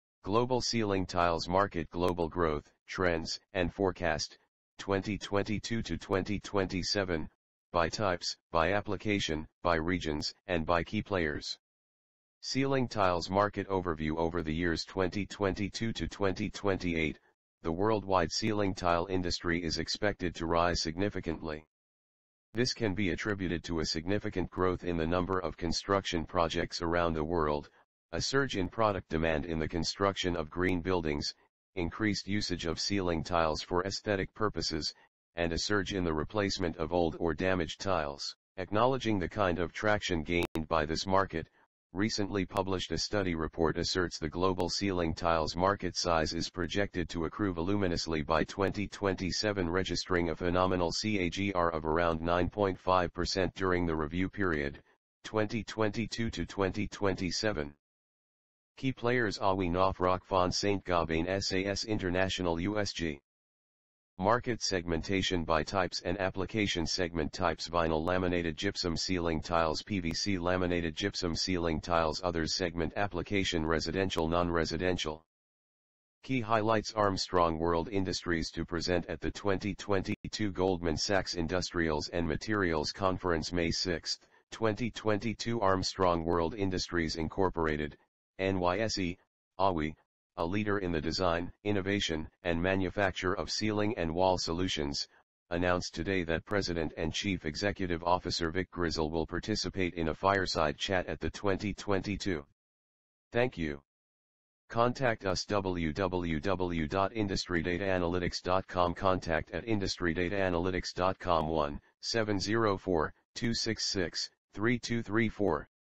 Global Ceiling Tiles Market| Industry Data Analytics | IDA
IndustryDataAnalytics Latest Published Global Ceiling Tiles Market - Global Growth, Trends and Forecast (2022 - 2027) By Types, By Application, By Regions and By Key Players AWI, Knauf, Rockfon, Saint-Gobain, SAS International, USG

Acknowledging the kind of traction gained by this market, recently published a study report asserts the global Ceiling Tiles Market size is projected to accrue voluminously by 2027 registering a phenomenal CAGR of around 9.5% during the review period (2022 to 2027).